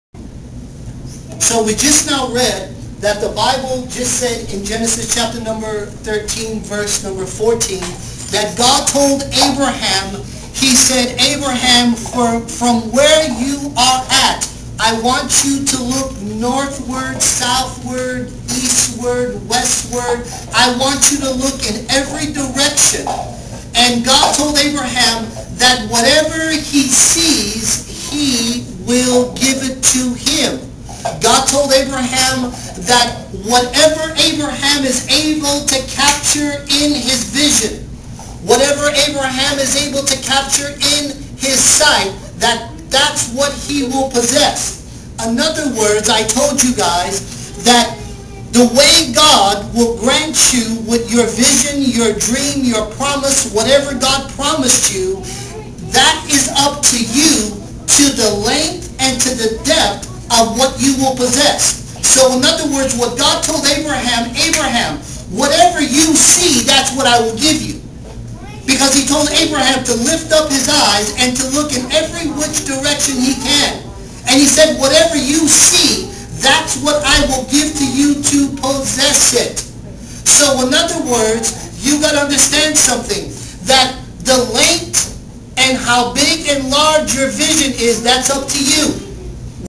20 20 Vision
Bishop Robert Stevens on how big your vision is